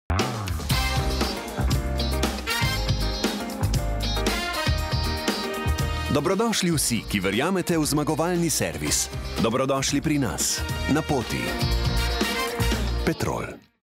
Zmagovalni servis ponovno z vami!
Dobrodošli vsi, ki verjamete v zmagovalni servis. Tako na odbojkarskem parketu, kot med našimi policami, kjer vas čaka sprejem s celovito ponudbo in vstopnicami za svetovno prvenstvo v odbojki.

Se vidimo v Stožicah!